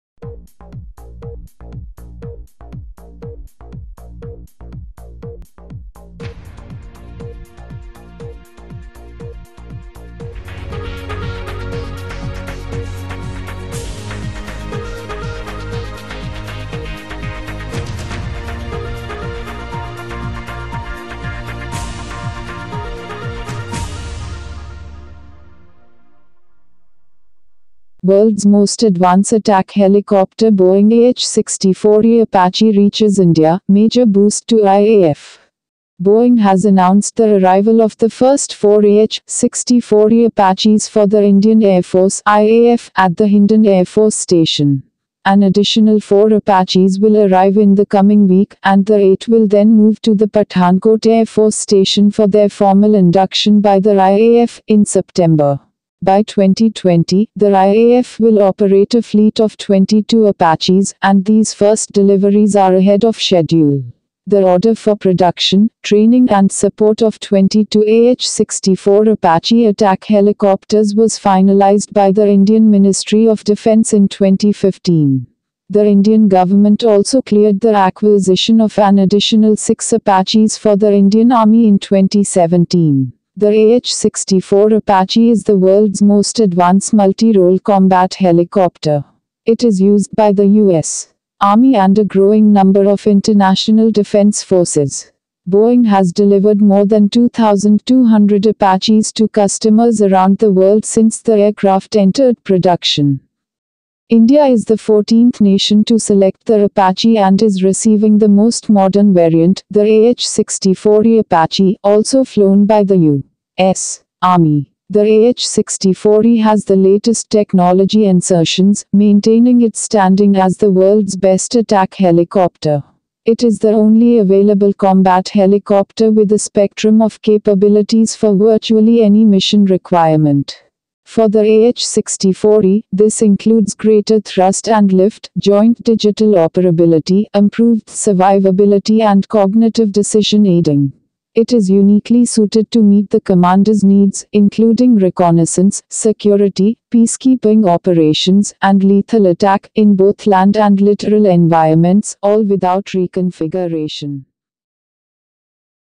World's Most Advanced Attack Helicopter Boeing AH-64E Apache Reaches India, Major Boost to IAF
Boeing has announced the arrival of the first four AH-64E Apaches for the Indian Air Force (IAF) at the Hindan Air Force Station. An additional four Apaches will arrive in the coming week, and the eight will then move to the Pathankot Air Force Station for their formal induction by the IAF, in September.

By 2020, the IAF will operate a fleet of 22 Apaches, and these first deliveries are ahead of schedule. The order for production, training and support of 22 AH-64 Apache attack helicopters was finalized by the Indian Ministry of Defence in 2015. The Indian government also cleared the acquisition of an additional six Apaches for the Indian Army in 2017